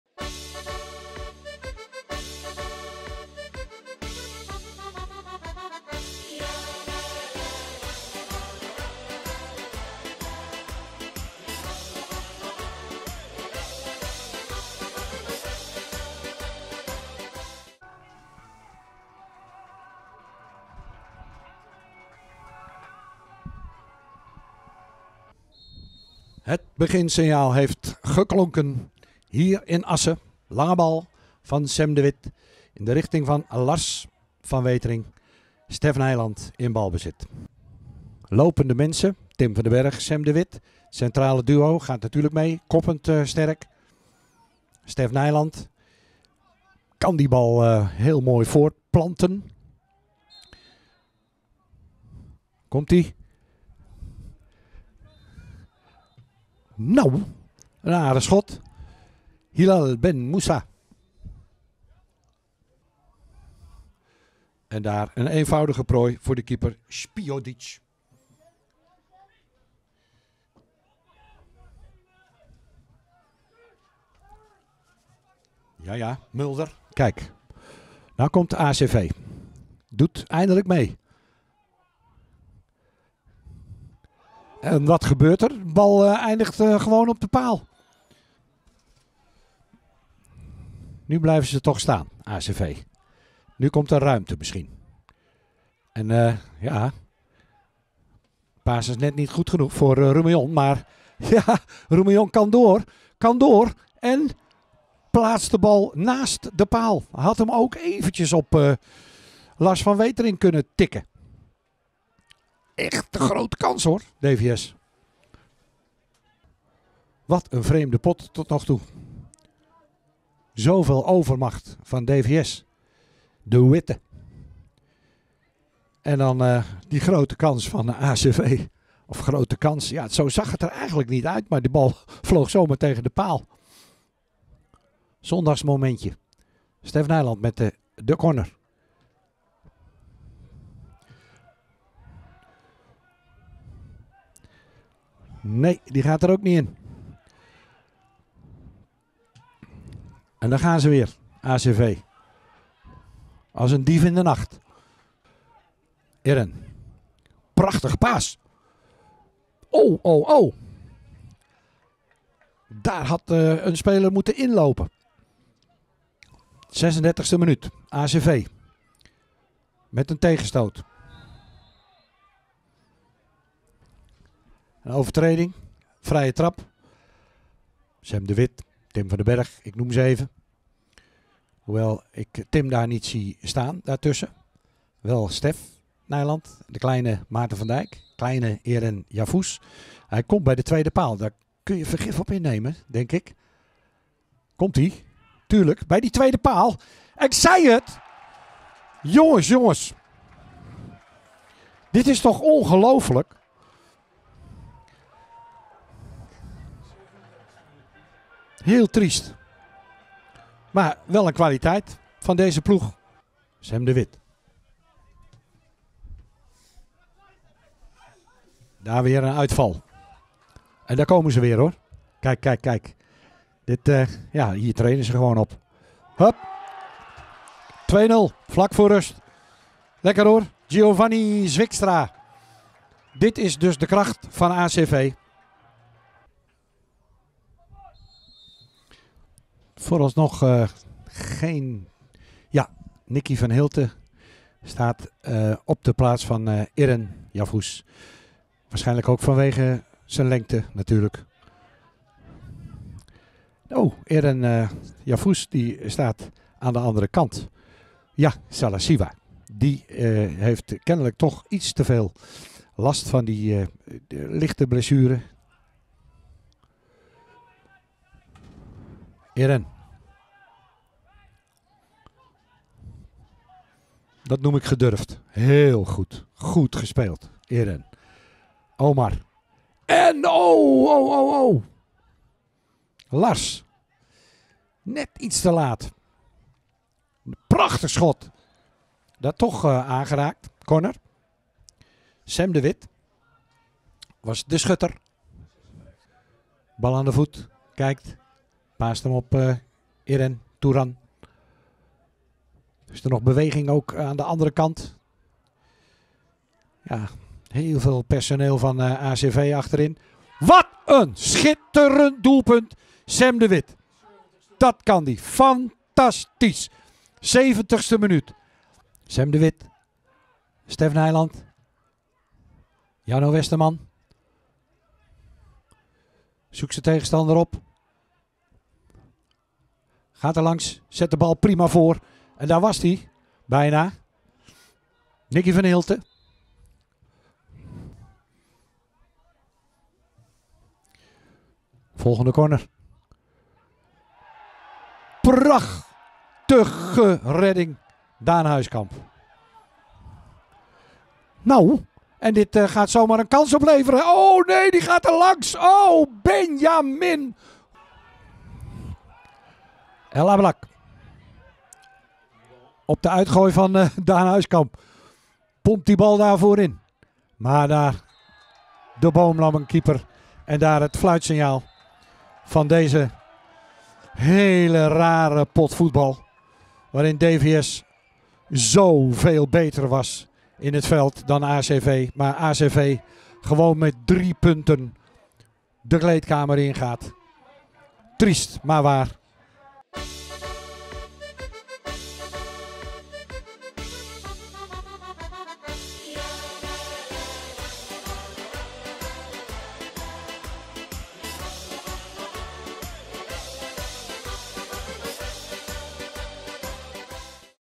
GEEN PUNTEN IN ASSEN | ACV ASSEN - DVS'33 ERMELO | Derde Divisie | Samenvatting
DVS'33 Ermelo verliest de uitwedstrijd tegen ACV Assen met 2-1.